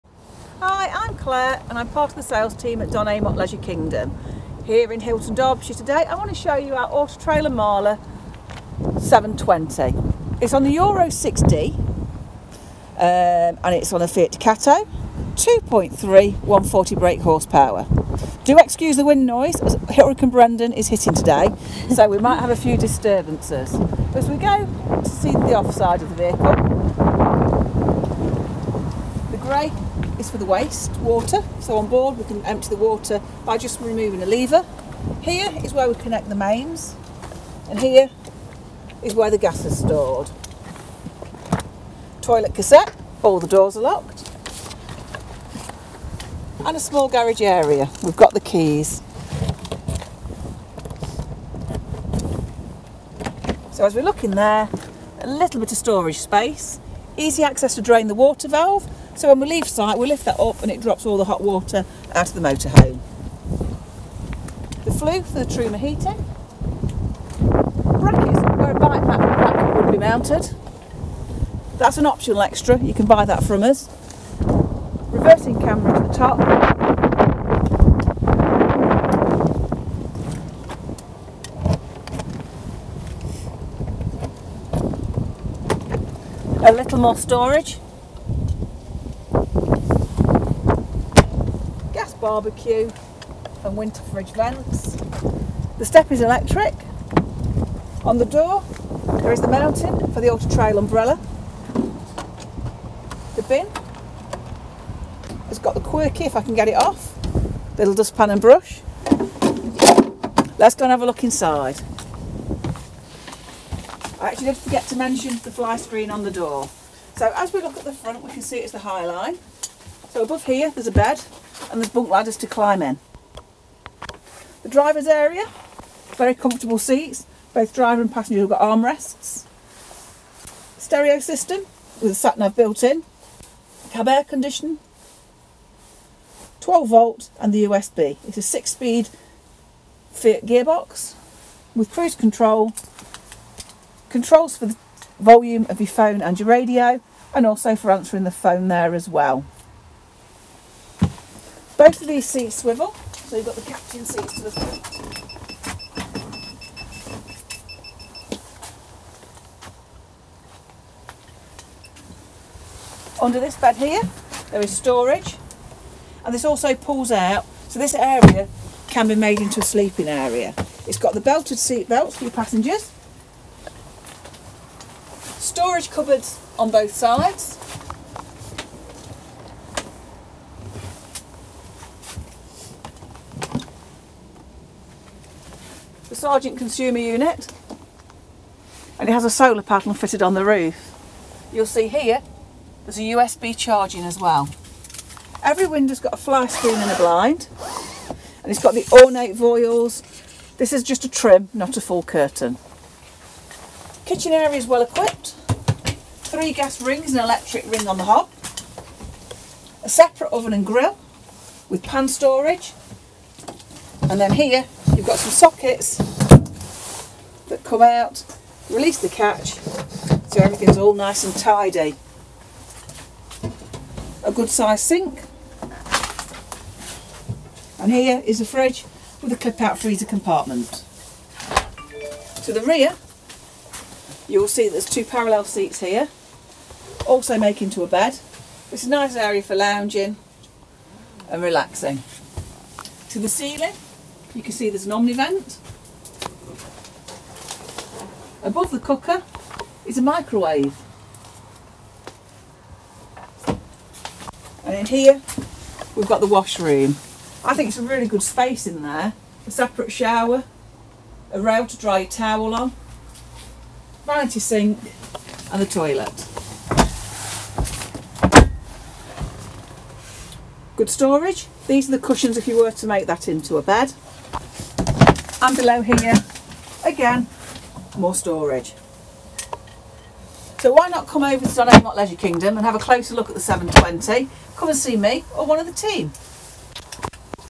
Auto-Trail Imala 720
** BRAND NEW 2020 MODEL ** 2020 welcomes four new models into the Imala range, two of these are HB versions which have a large 1.15m tall storage compartment at the rear, perfect for families who like to take bulky sports equipment or alfresco dining furniture on their travels.
This model is the Imala 720 Hi Line in White. It is built on the new Fiat Ducato Euro 6D 2.3 litre upgraded to a 140 bhp engine with a 6-speed manual gearbox and with 'Media Pack' fitted.
The 720 comes with 4 belts and 4 berths which makes it an ideal family holiday motorhome and this model as it is the high line, sleeps 6. There are two lounge areas as well as a spacious side washroom. There is also a spacious galley kitchen that comes fully equipped. You ll be hitting the road with stylish 16 glossy Fiat alloy wheels, as well as beautiful and contemporary wood coloured furniture. Charging up the family portable devices will be no problem as modern USB ports are located within the habitation area and the new Truma 4KW saloon/water heating includes an enroute regulator, so you ll be able to heat the habitation area whilst on the move.
Come along to our Showground to view this fantastic for 2020 Auto-Trail Imala 720 Hi Line. PRICE SUBJECT TO REGISTRATION BEFORE 31 MARCH 2020!